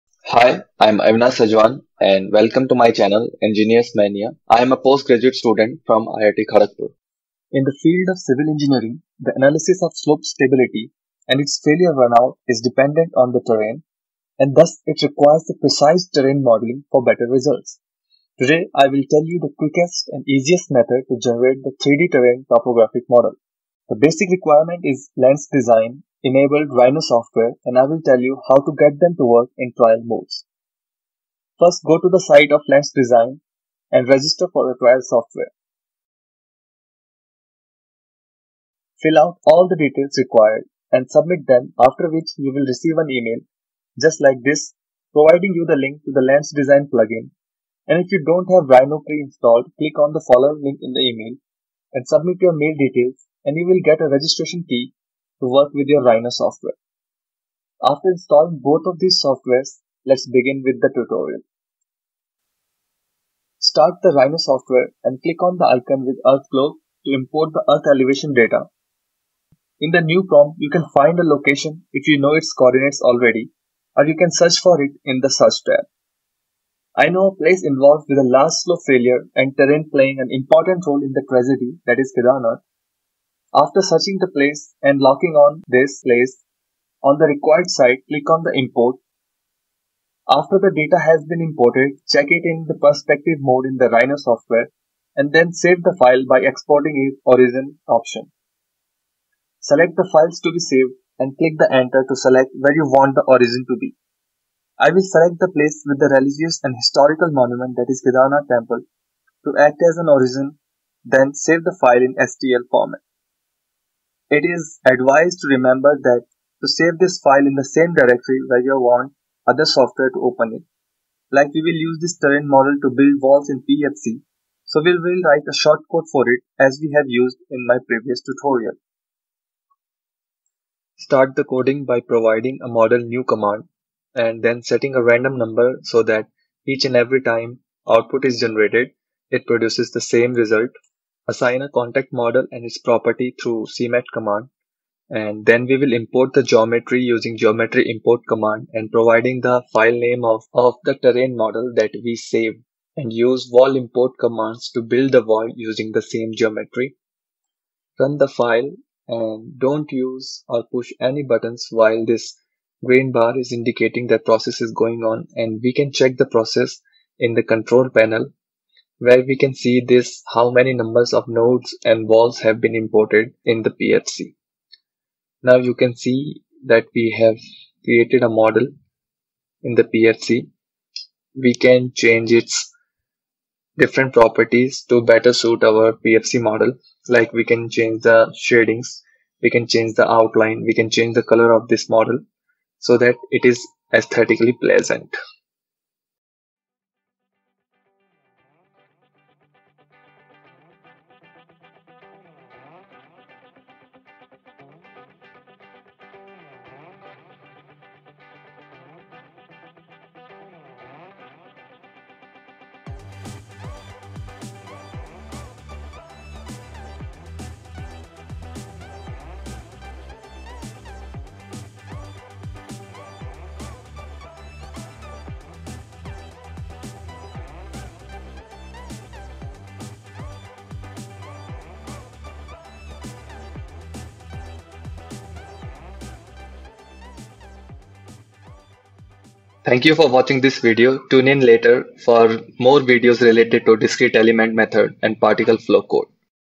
How to create a 3D terrain-topographical model using "Rhino" and "Lands design".
There are multiple ways to import the earth elevation data in terms of contours, topographical maps, and 3d models using Blender, SketchUp, and Google Earth. All these methods are exhausting and not very beginner-friendly.
So this video is for all types of people to quickly and easily generate an accurate and precise 3d model of any terrain without spending any money on the pro software.